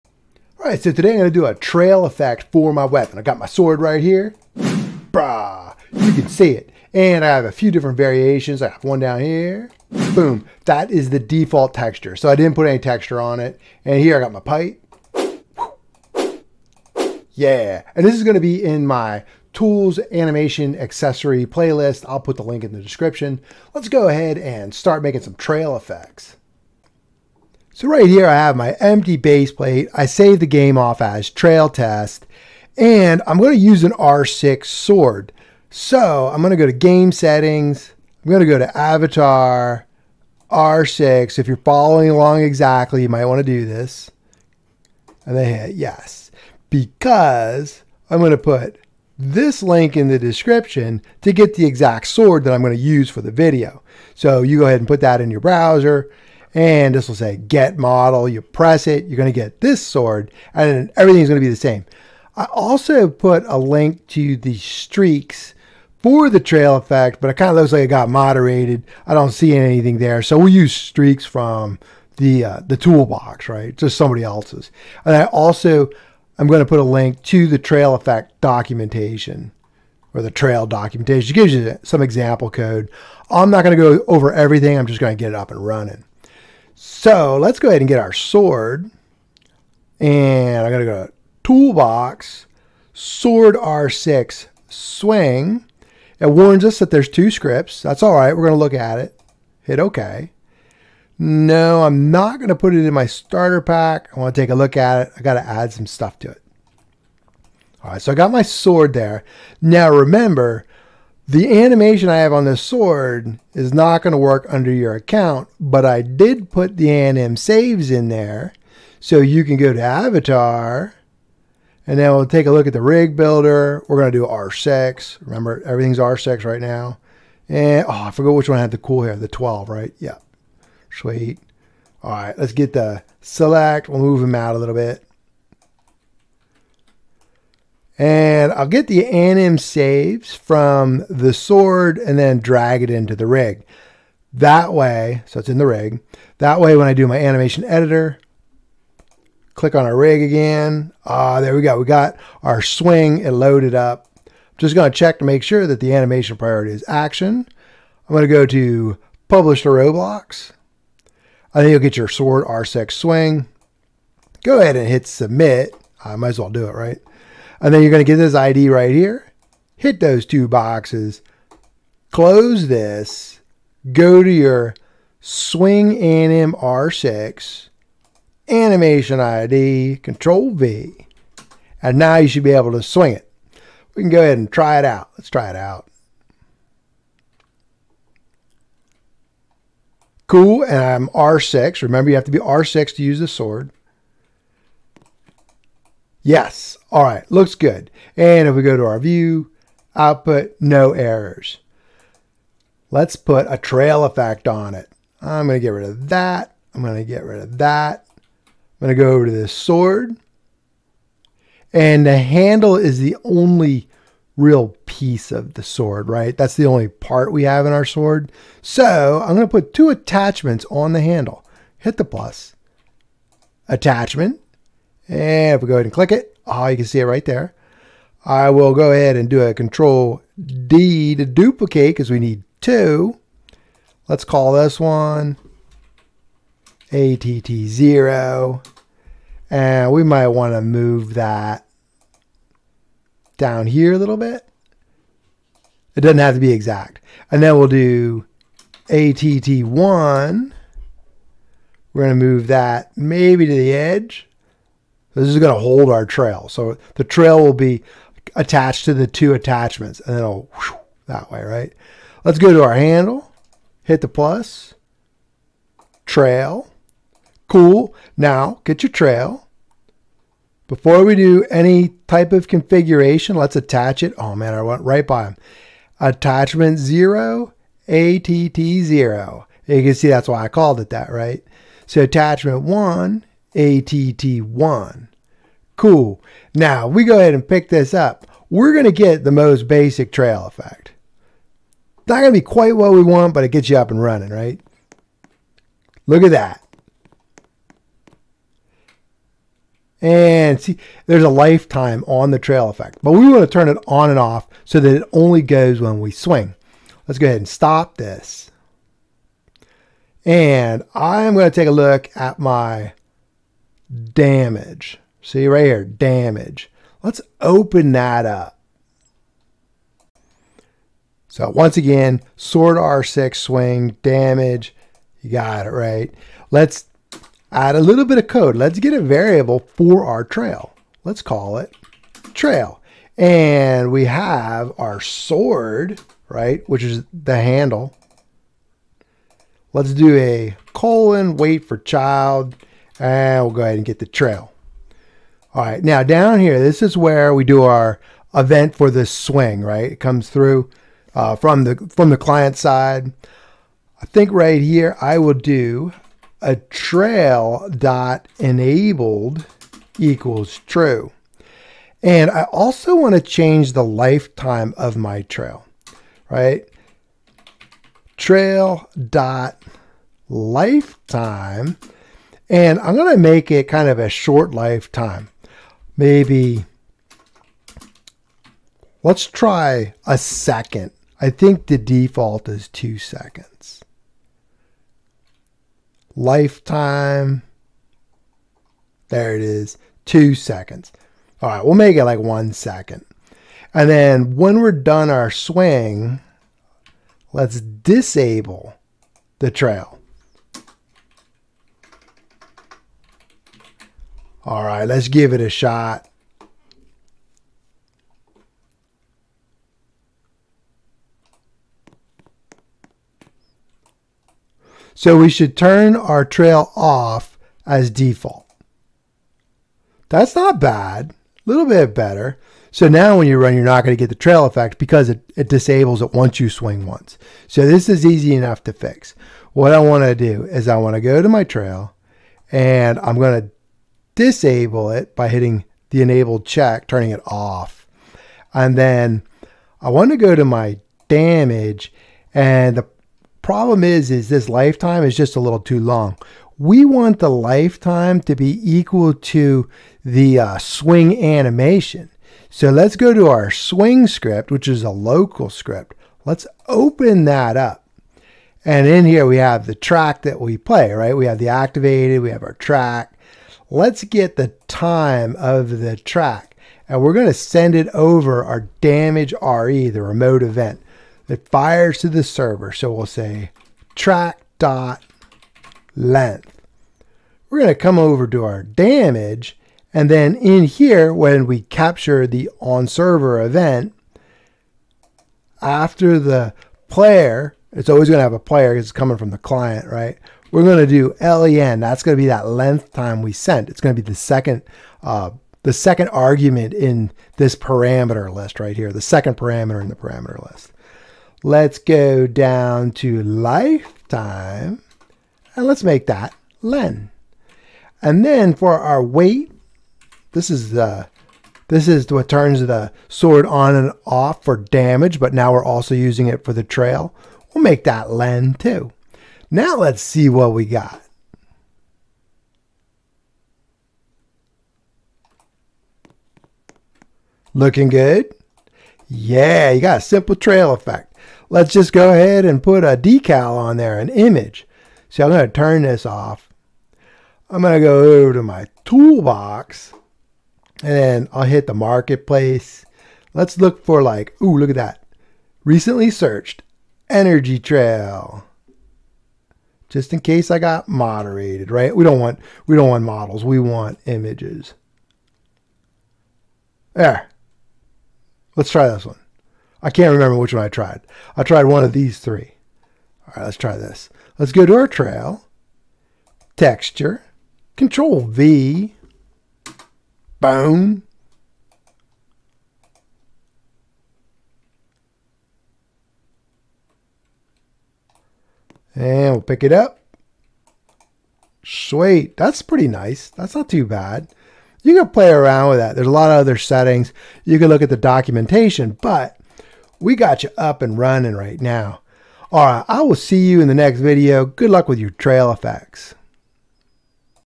How to Add a Trail Effect | Roblox Studio | 2023
Trail effects for a swingable weapon